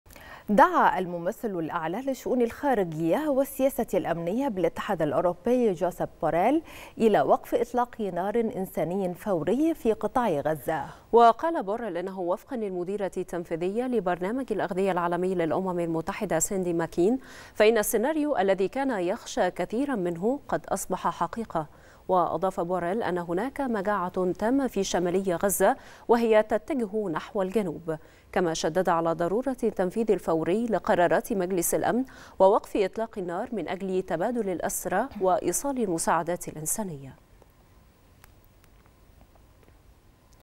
بوريل يدعو إلى وقف فوري إنساني لإطلاق النار في غزة
نشرة الثالثة | بوريل يدعو إلى وقف فوري إنساني لإطلاق النار في غزة